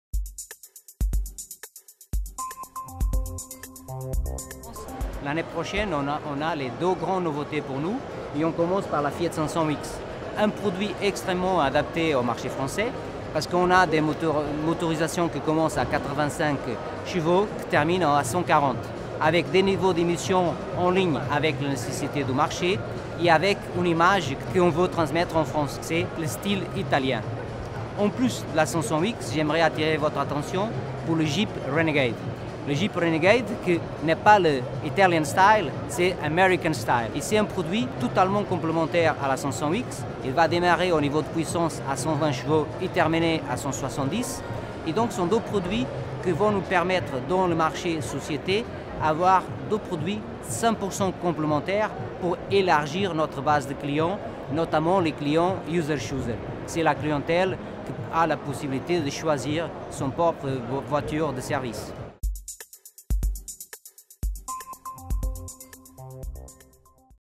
Fiat au Mondial 2014 : quelles nouveautés pour les flottes ?
José Carreira, directeur des ventes entreprises et véhicules d'occasion Fiat France, présente la Fiat 500X avec des motorisations de 85 à 140 ch, ainsi que le Jeep Renegade, produit complémentaire à la 500X, avec une puissance minimum de 120 ch.